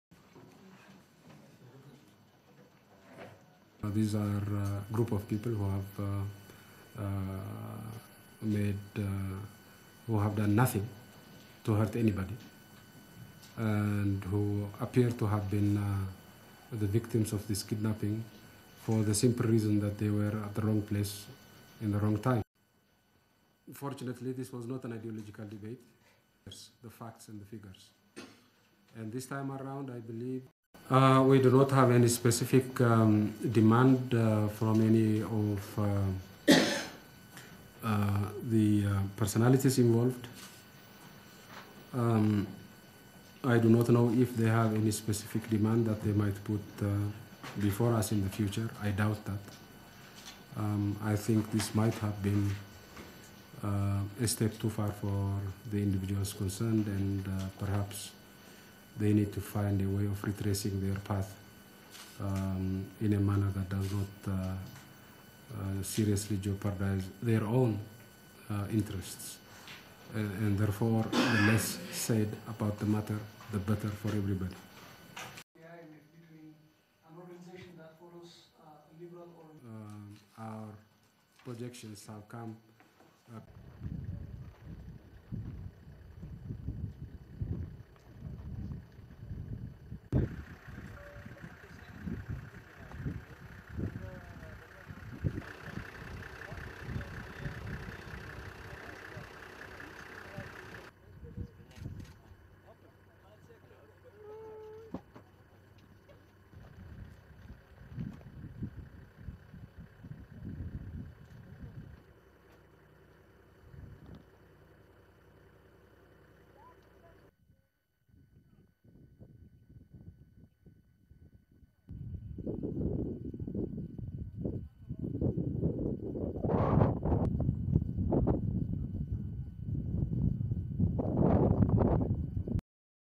WRAP PM says missing Europeans were "at the wrong place, in the wrong time"; ADDS bite
Addis Ababa, March 12 2007
1. Wide shot of news conference
2. SOUNDBITE: (English) Meles Zenawi, Ethiopian prime minister:
"These are a group of people who have made, who have done nothing to hurt anybody and who appear to have been the victims of this kidnapping for the simple reason that they were at the wrong place in the wrong time."
3. Various of reporters
4. SOUNDBITE: (English) Meles Zenawi, Ethiopian prime minister:
"We do not have any specific demand from any of the personalities involved. I do not know if they have any specific demand they might put before us in the future, I doubt that. I think that this might have been a step too far for the individuals concerned, and perhaps they need to find a way of retracing their path in a manner that does not seriously jeopardise their own interests. And therefore the less said about the matter the better for everybody."
5. Cutaway of reporter's notebook
6. Cutaway of camera crew
Hamedali, Afar region, Recent
7. Afar villagers with guns in foreground, journalists' vehicles in background (not connected to abduction)
8. Close-up of tour company logo on side of vehicle involved in attack and abduction
9. Bullet holes on front passenger door
10. Close-up of luggage in back of vehicle
11. Wide shot of bullet ridden car with door open
12. Close-up of luggage in back of vehicle
13. Close-up of number plate
14. Various of abandoned pick up truck
STORYLINE:
Five Europeans abducted in one of Ethiopia's most remote, inhospitable regions, were not personally targeted, the prime minister said in the capital Addis Ababa on Monday.
The group, all of whom are linked to the British Embassy in Addis Ababa, were in the "wrong place at the wrong time," Meles Zenawi said in his first comments on the kidnapping since they were seized 12 days ago.
"We do not have any specific demand from any of the personalities involved. I do not know if they have any specific demand they might put before us in the future," Meles added.
Meles refused to go into details on where the hostages were being held, saying he did not want to jeopardise their safety.
The hostages, three British men, an Anglo-Italian and a French woman, were on a tourist trip to the remote Afar region of northeastern Ethiopia when they were taken captive.
They were seized at gunpoint with 13 Ethiopians. Five of the Ethiopians are believed to have escaped or been released.
Their Toyota Land Cruiser and a Land Rover Discovery, which had diplomatic plates, were discovered on March 5 riddled with bullet holes and grenade shrapnel.
The 4x4s still contained the victims' luggage, as well as shoes and mobile phones, when they were found in Hamedali, the last staging post before the Afar region's famous salt lakes.
The items found inside suggested robbery was not a motive.
While the region's ancient salt mines and volcanoes offer a spectacular, moonlike landscape, bandits operate in Afar and tourists must have armed guides.
Afar is also close to the disputed border between Ethiopia and archrival Eritrea, an area littered with land mines from numerous conflicts in the region.